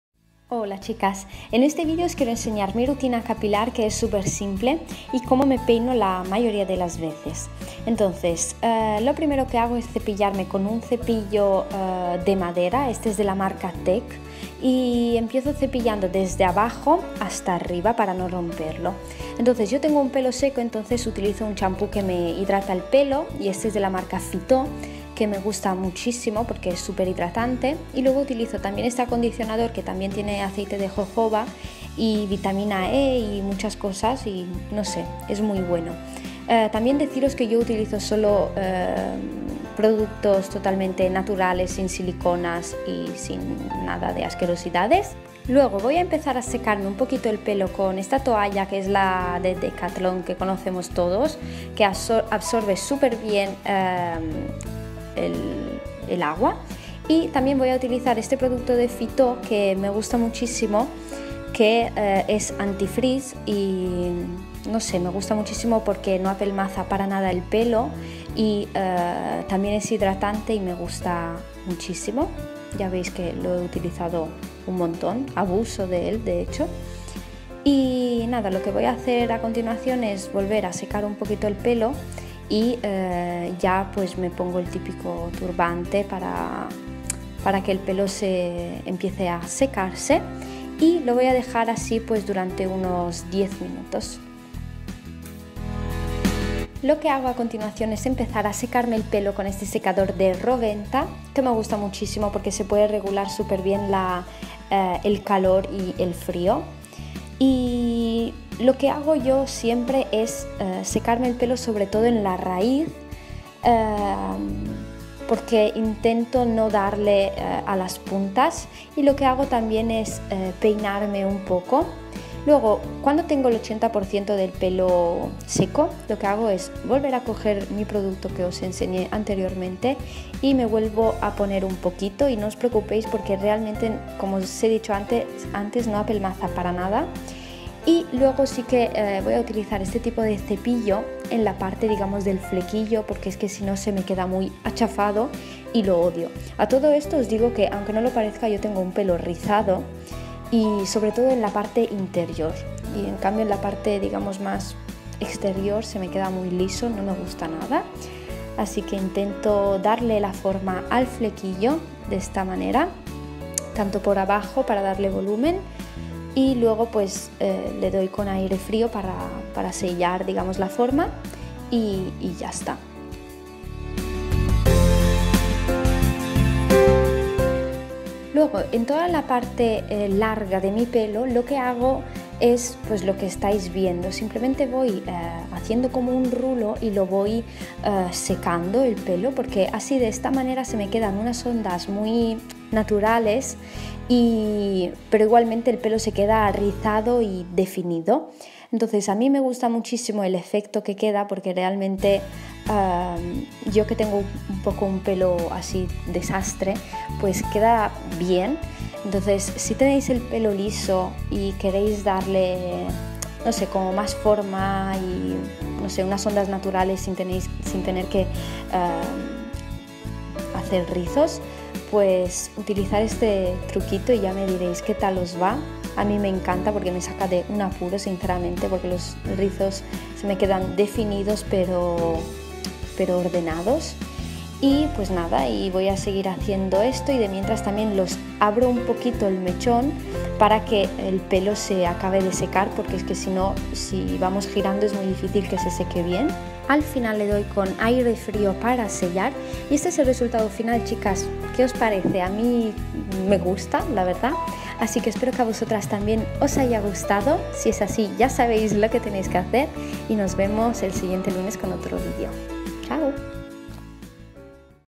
Hair routine: Pelo brillante y sin encrespamiento
En este vídeo os enseño cómo me seco el pelo y cuáles son mis productos que me ayudan a mantenerlo hidratado y sobre todo como me lo seco para no tenerlo encrespado, es decir sin frizz! Si queréis ver más vídeos relacionados con mi pelo o peinados dejármelo en los comentarios! Besitos!